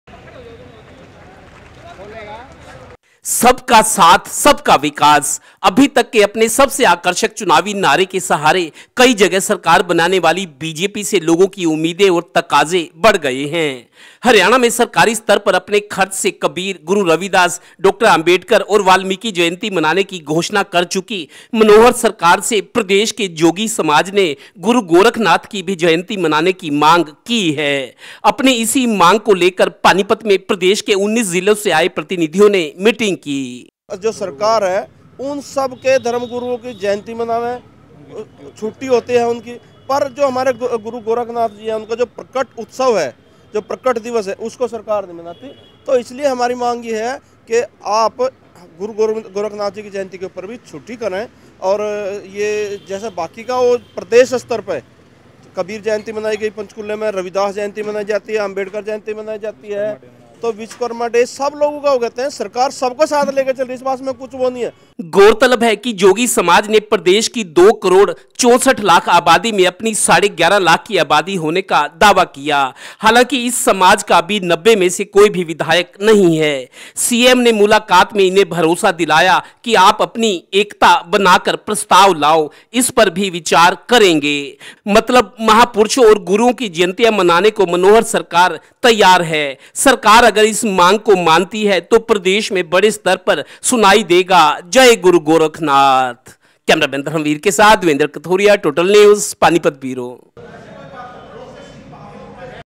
'Jogi Samaj' demands for 'Guru Gorakhnath' Jayanti
Panipat, February 12: Jogi Community led a meeting in order to demand for the 'Jayanti of Guru Gorakhnath.
Community wants the government to announce the Jayanti Celebration on 'State Level'.